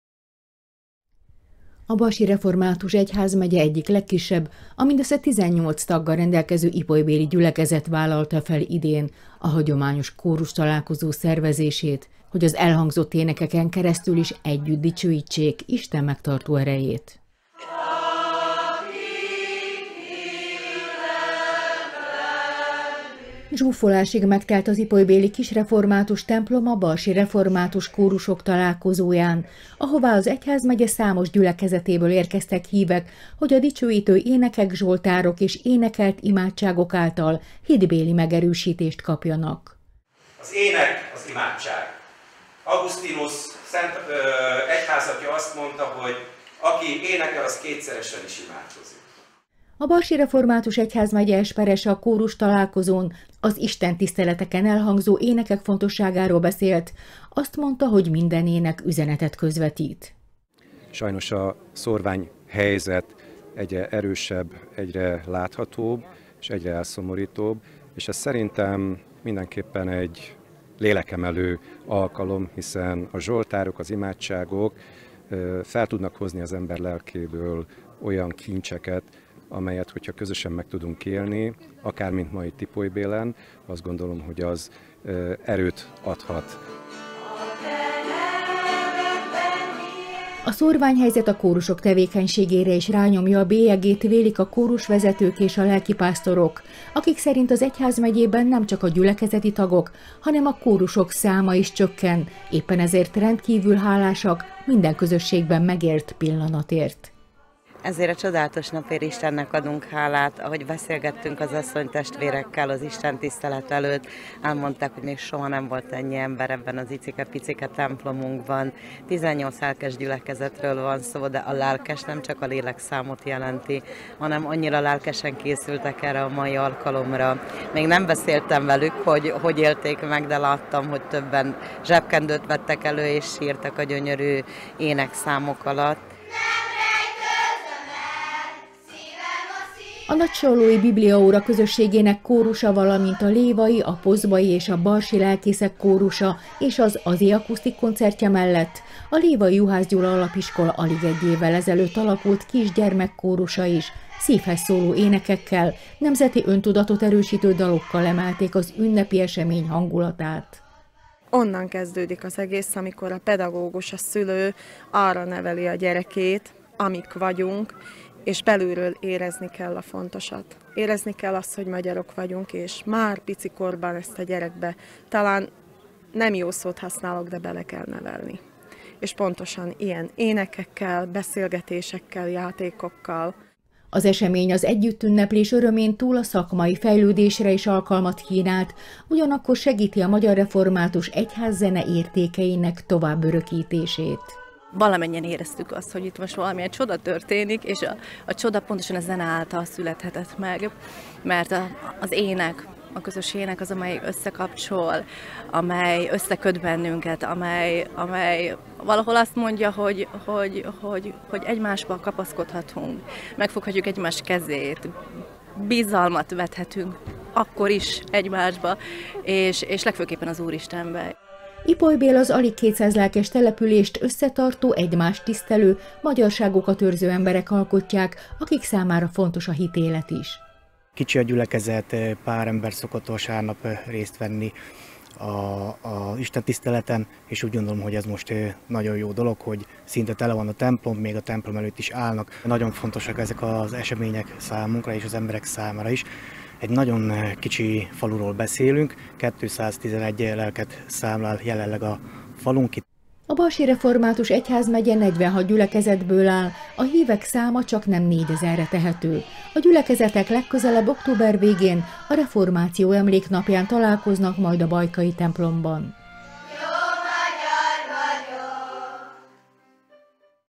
Egyházmegyei kórustalálkozó a barsi reformátusok legkisebb gyülekezetében
Ipolybél. A Barsi Református Egyházmegye egyik legkisebb, a mindössze 18 taggal rendelkező ipolybéli gyülekezet vállalta fel idén a hagyományos kórustalálkozó szervezését, hogy az elhangzott énekeken keresztül közösen dicsőítsék Isten megtartó erejét.